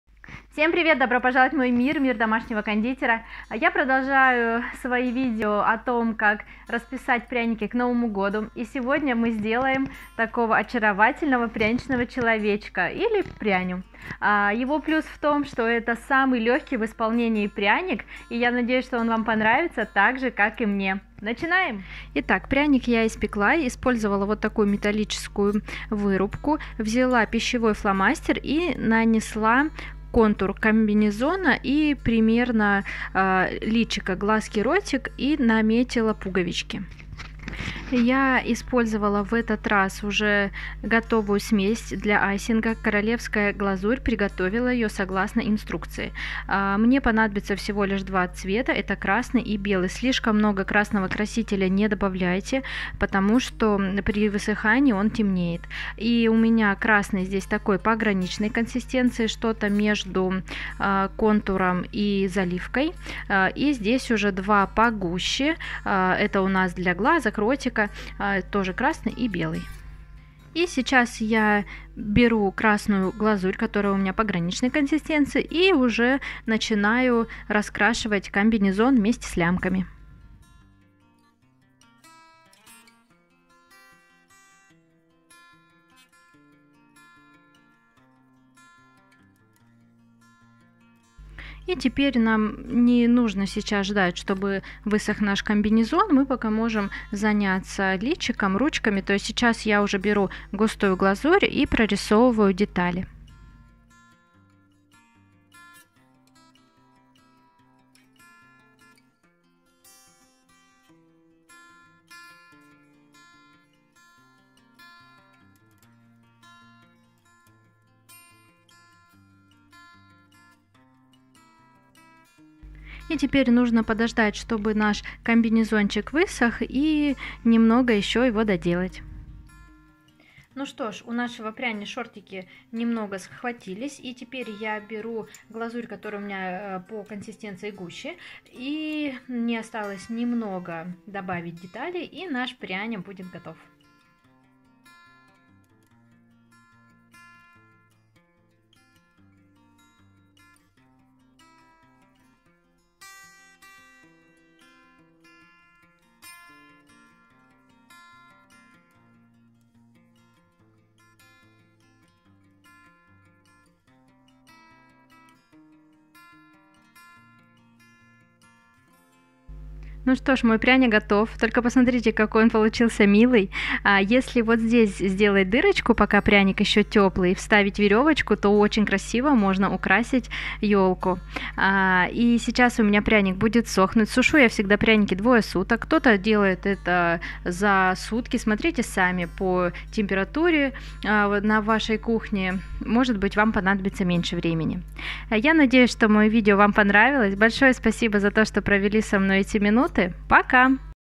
Новогодние пряники. Имбирный пряник-человечек.
В этом видео я покажу как сделать новогоднего пряничного человечка.